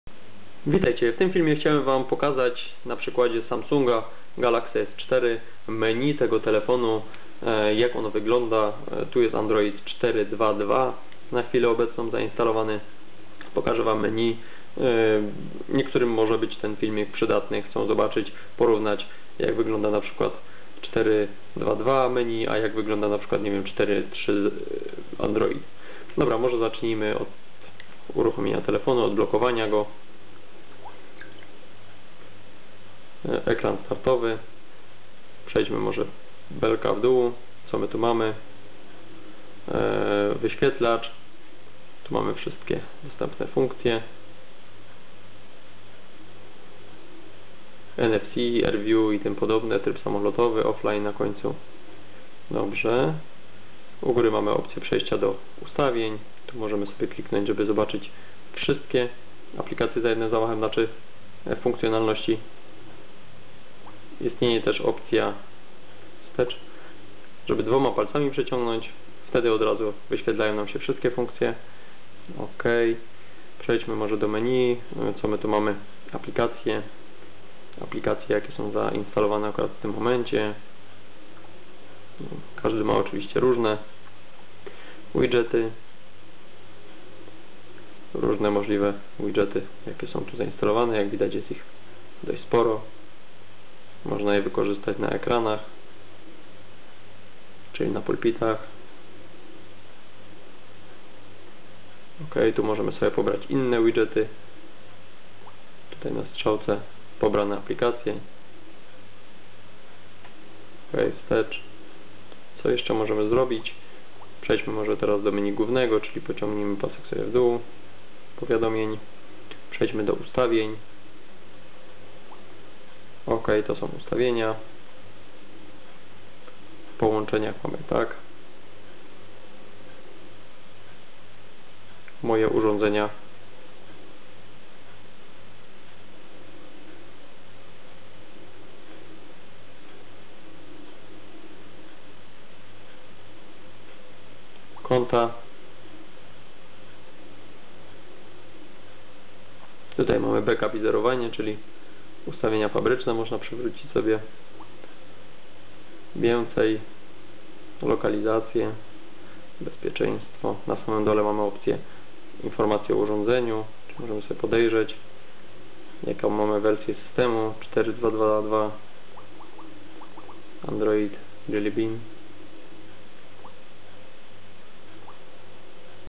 Samsung Galaxy S4 MENU Android 4.2.2 | ForumWiedzy
Coś niejasne? Pytaj! Odpowiemy natychmiast! Something vague? Ask, answer instantly!
Przegląd menu telefonu Galaxy S4  android 4 2 2
Overview of the phone menu Galaxy S4

Dołącz do ForumWiedzy w mediach społecznościowych i bądź na bieżąco!